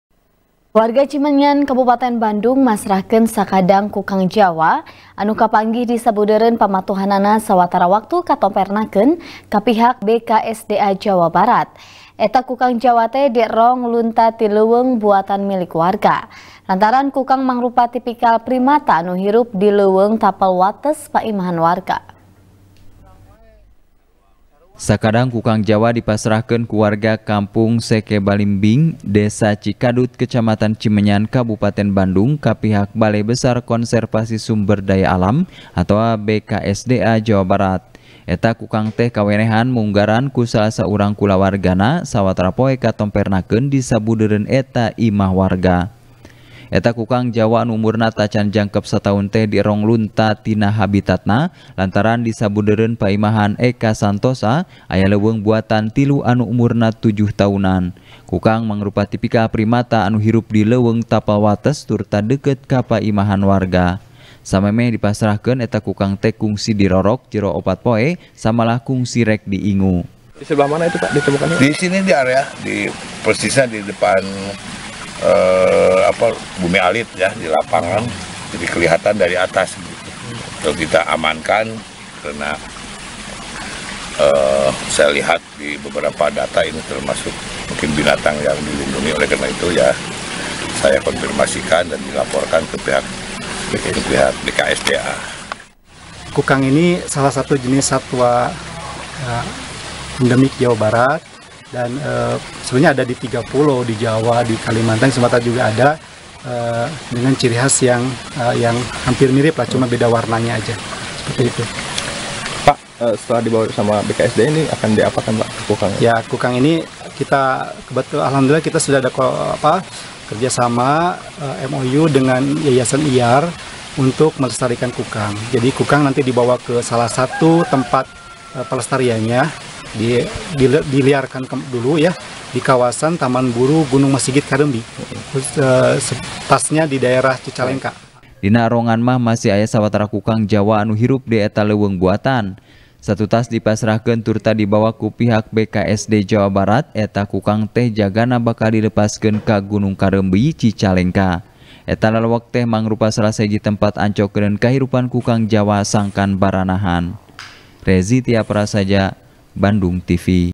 Kukang Jawa Ngulayaban ka Paimahan Warga | TP | BANDUNGTV
TP | BANDUNGTV | ASTRI CAHYANI | Sakadang kukang jawa, dipasrahkeun ku warga kampung Sekebalimbing, desa Cikadut, kecamatan Cimenyan, kabupaten Bandung, ka pihak Balai Besar Konservasi Sumber Daya Alam, atawa BKSDA Jawa Barat. Eta kukang jawa teh, kawenehan munggaran ku salasaurang kulawargana, sawatara poe katompernakeun di sabudeureun eta imah warga.